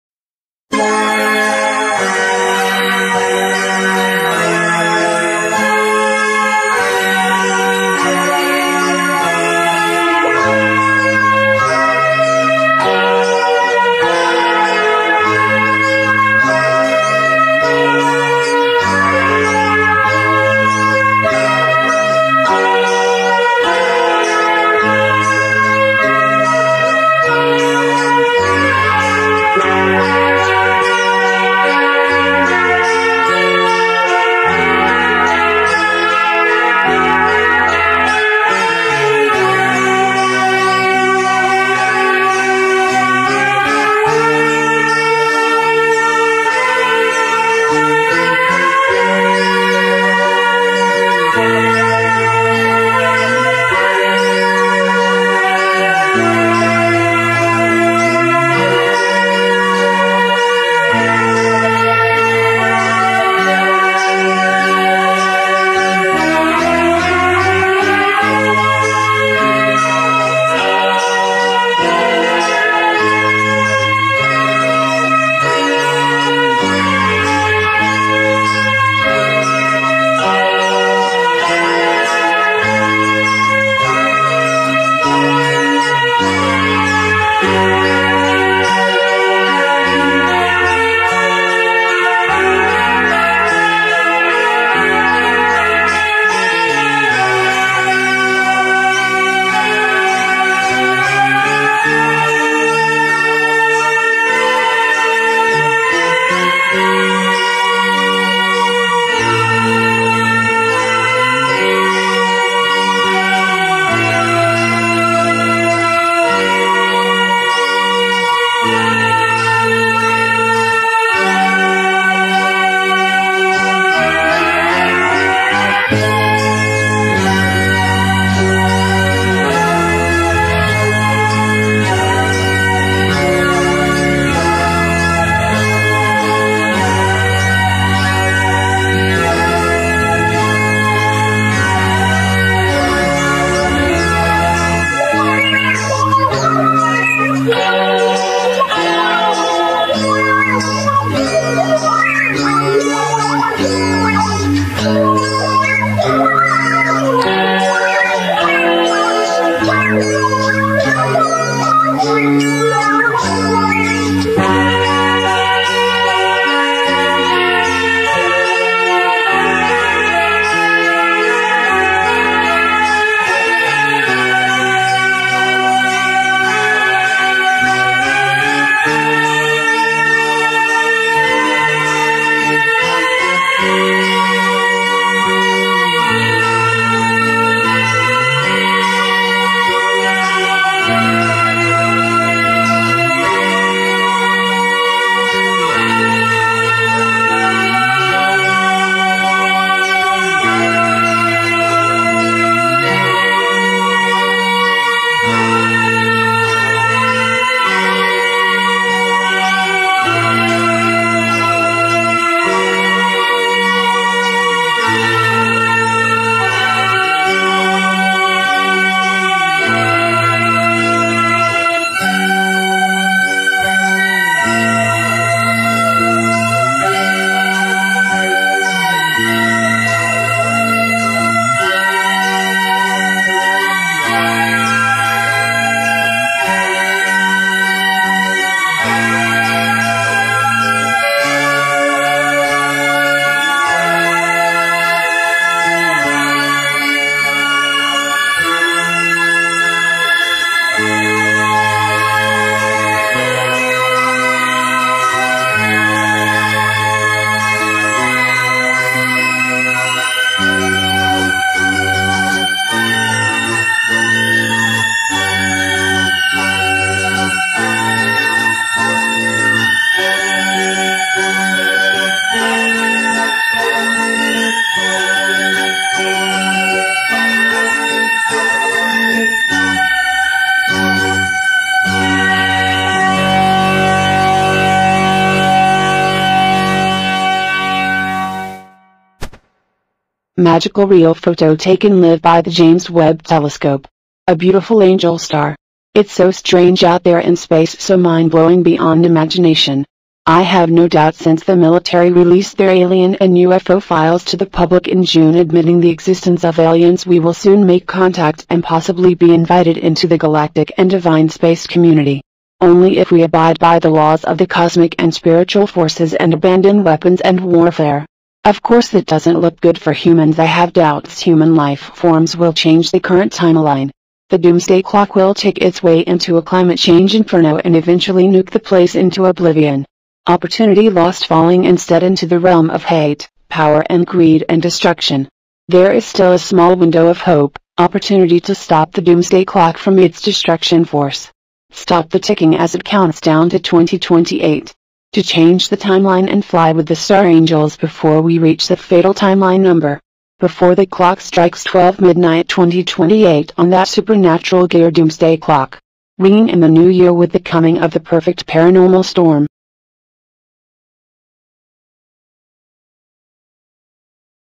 Star Angel caught live by James Web Telescope - Lenny Lionstar
Beautiful images beyond imagination being captured by the James Web Telescope. A Star Angel has been caught live flying through the universe.